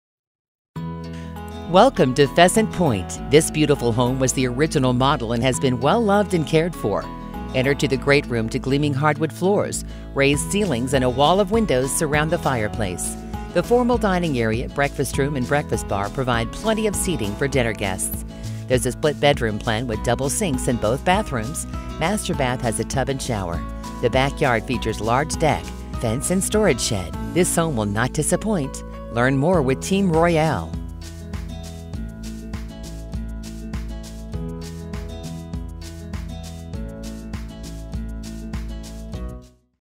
2015 Gosling Court Franklin, IN | MLS# 21590368
2015 Gosling Court, Franklin, IN

Welcome to Pheasant Pointe! This beautiful home was the original model and has been well loved and cared for. Enter to the great room to gleaming hardwood floors, raised ceilings, and a wall of windows surround the fireplace. Formal dining area, breakfast room and breakfast bar provide plenty of seating for dinner guests. Split bedroom plan with double sinks in both bathrooms, Master Bath has tub and shower. Backyard features large deck, fenced and storage shed. This home will not disappoint!